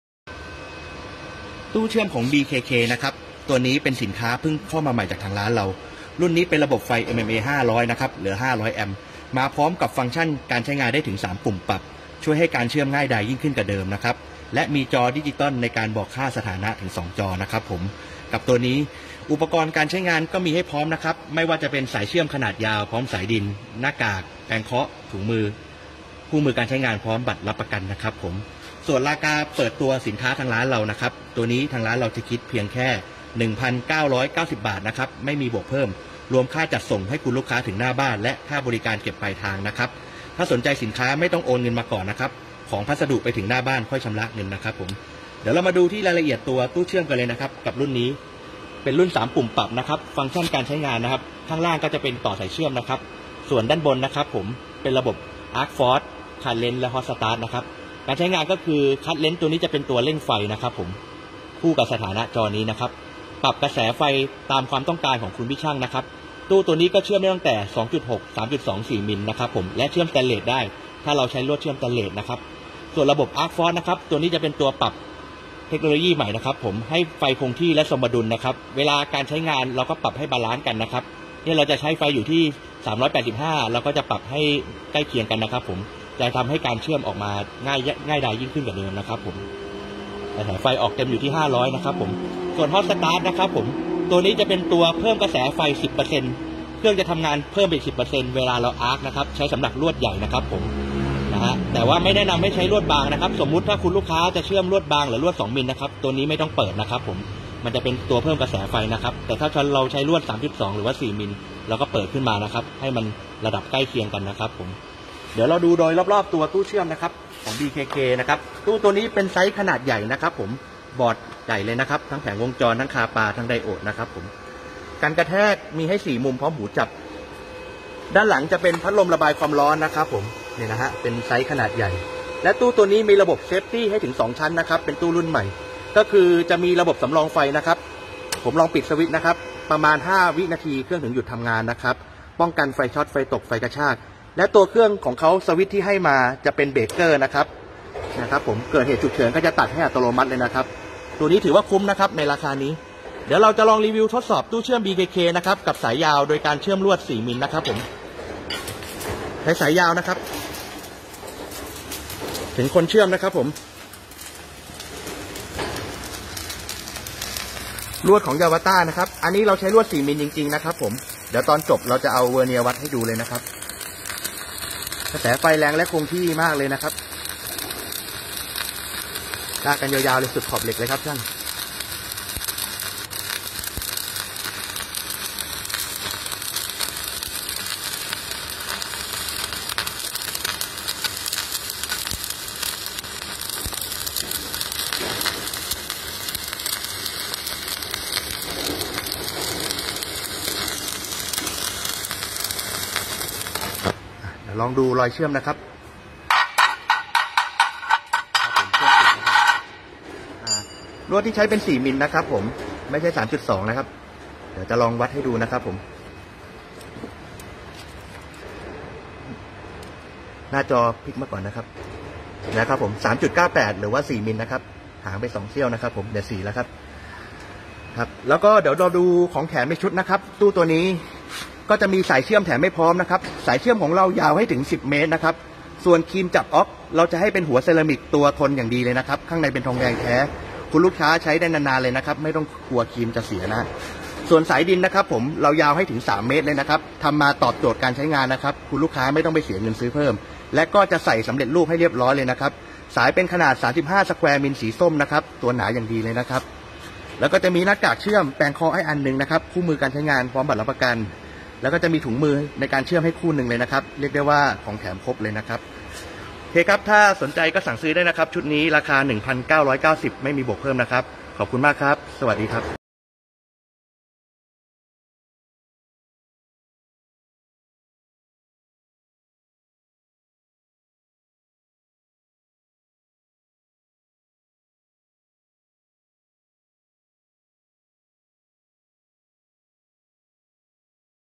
🔴ตู้เชื่อม BKK MMA 500 แอมป์ จอดิจิตอล 3 ปุ่มปรับ พร้อมสายเชื่อมยาว 10เมตร
พิกัด ตู้เชื่อม BKK คุณภาพสูง

ใหม่ล่าสุด!!! ตู้เชื่อม BKK MMA 500 แอมป์ จอดิจิตอล 3 ปุ่มปรับ พร้อมสายเชื่อมยาว 10เมตร
ราคา 1,990 บาท ส่งฟรี เก็บเงินปลายทาง‼️
- รุ่นขนาดใหญ่
- มีกันกระแทก 4 มุม พร้อมหูจับ
- มีระบบ เซฟซี้ 2 ชั้น สำรองไฟได้ และ มีเบรคเกอร์
- พัดลมระบายความร้อน
- สายเชื่อมยาว ยาว 10 เมตร สายดิน 3 เมตร
- คิมจับอ๊อก หัวเซรามิค
- หน้ากากเชื่อม
- ถุงมือ 1 คู่
- แปรงเคาะ 1 อัน
- เชื่อมทั้ง เหล็ก แสตนเลส

เเรงดันกระเเสไฟจ่าย 28
ใช้เทคโนโลยี advance inverter และ IGBT คุณภาพ ทำให้ประหยัดไฟ
- อุปกรณ์ครบชุด มีปลั๊ก และ ใบรับประกันพร้อม
- ใช้งานง่าย เชื่อมนิ่ม เรียบสม่ำเสมอ แนวเชื่อมสวย และ กระแสไฟคงที่ต่อเนื่องตามค่าที่ปรับตั้ง
- มีพัดลมระบบระบายความร้อนภายในตัวเครื่อง เพื่อช่วยระบายความร้อนอย่างต่อเนื่อง และป้องกันความเสียหายเมื่อเครื่องใช้งานเกินภาระ
- มีระบบป้องกันภายในตัวเครื่อง ตัดการทำงานอัตโนมัติพร้อมสัญญาณแจ้งเตือนเมื่อเครื่องใช้งานเกินภาระ หรือ แรงดันไฟสูงกว่าปกติ
การใช้งาน
- ใช้ในงานติดตั้งภายนอกอาคาร ประกอบชิ้นส่วนโลหะ ประกอบเฟอร์นิเจอร์ ซ่อมแซมโครงสร้าง ประกอบเครื่องใช้ในครัวเรือน อู่ซ่อมรถยนต์
- เหมาะสำหรับงานสแตนเลส และ สแตนเลส เหล็ก